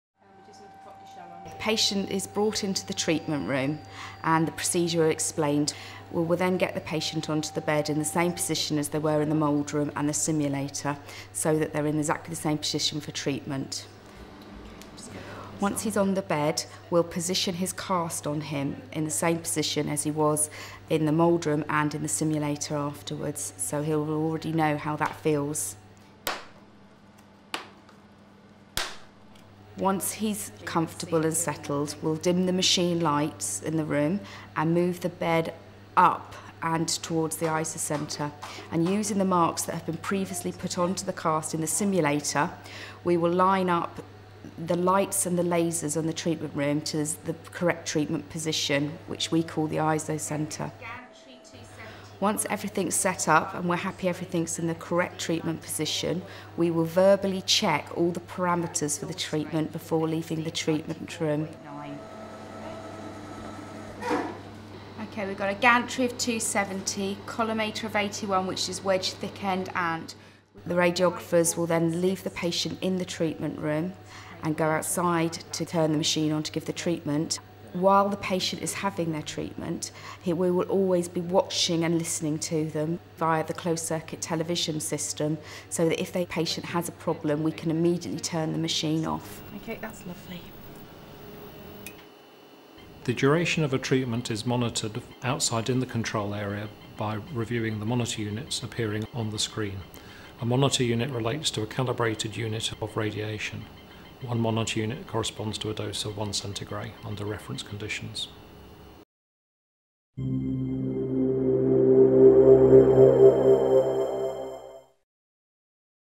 Treating the head and neck - Radiotherapy and its physics (3/15)
Targeting the head and neck for treatment of cancer.